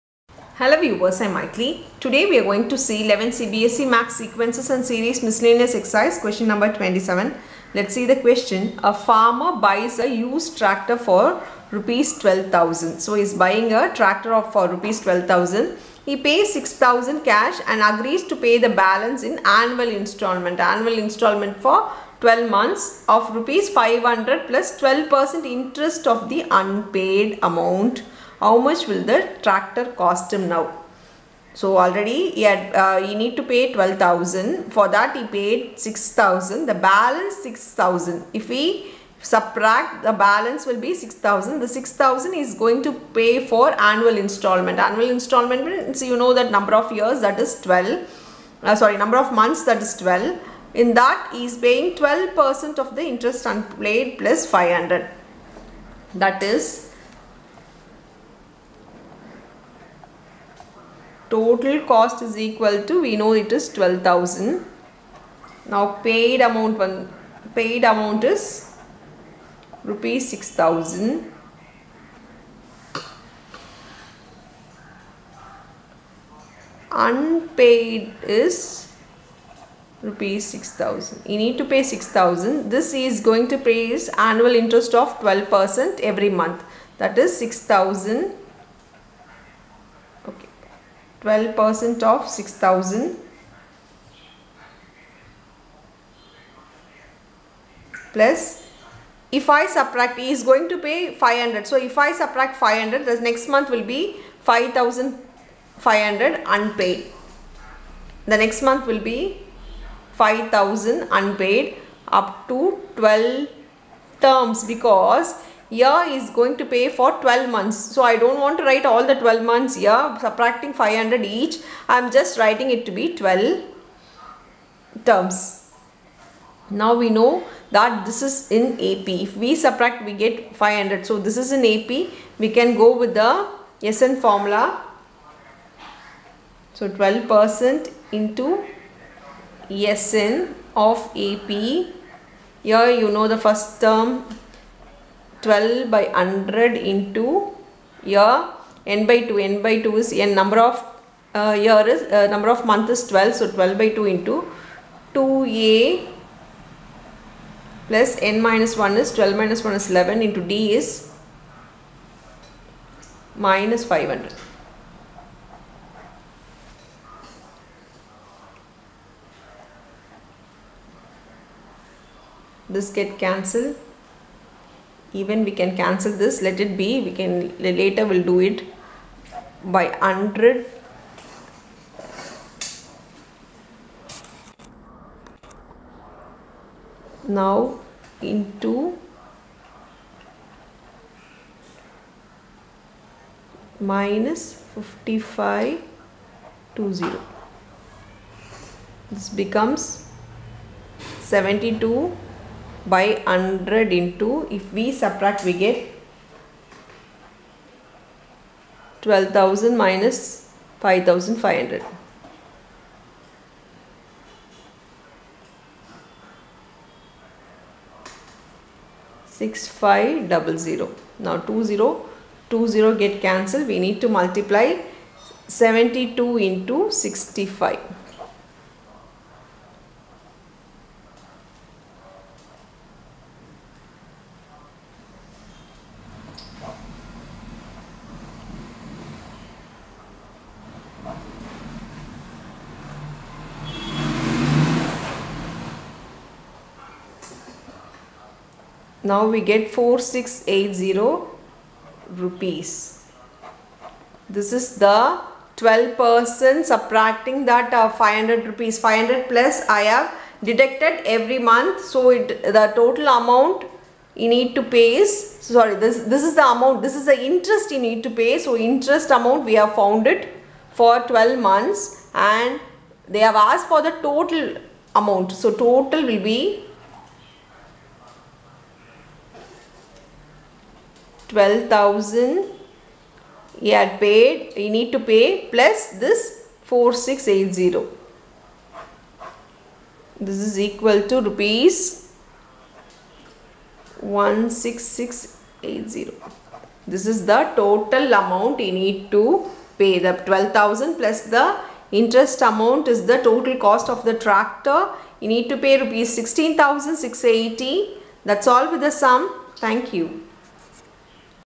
CBSE NCERT CLASS 11 MATHS SEQUENCES & SERIES MISCELLANEOUS EX SUM 27 (SOLUTION IN ENGLISH)
CLASS11 SEQUENCES&SERIES#